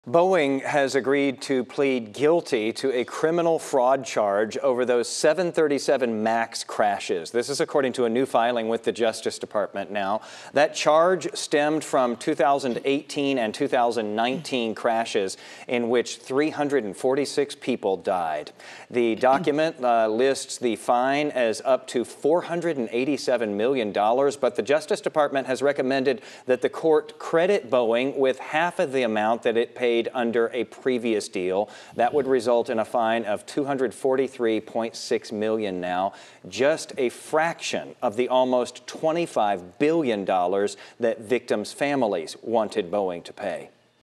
Boeing agrees to plead guilty to criminal fraud charge over 737 MAX crashes
The charges stem from crashes in 2018 and 2019 that resulted in the deaths of 346 people.